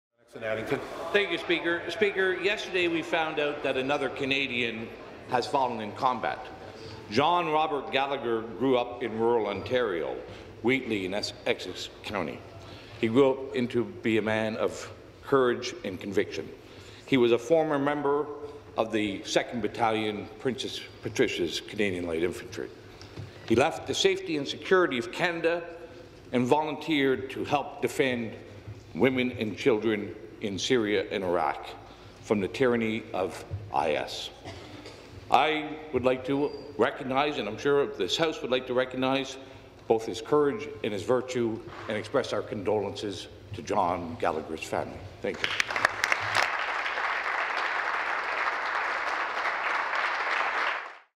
Hillier Honors John Robert Gallagher
MPP Randy Hillier spoke in the House to honor John Robert Gallagher, a Canadian who gave his life in combat defending people from the tyranny of ISIS.